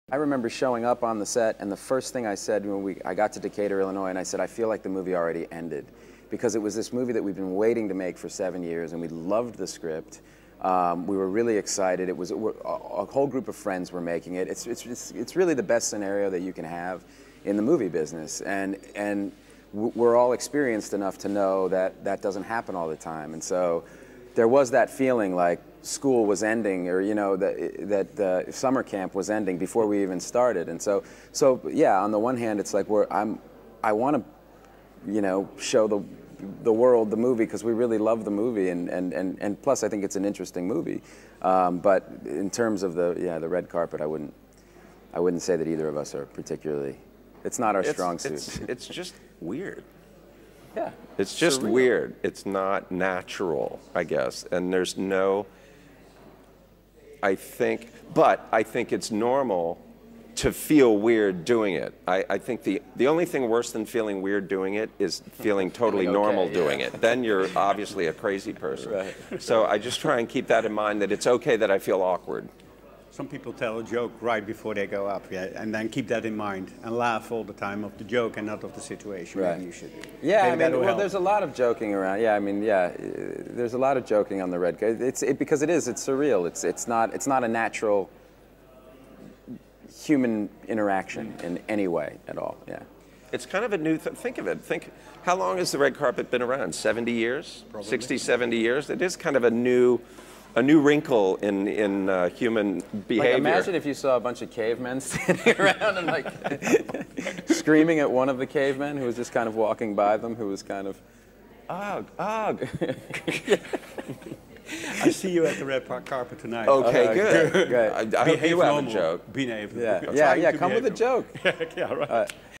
66th Venice Film Festival - Matt Damon - Steven Soderbergh
Matt Damon and Steven Soderbergh talk about "The Informant!".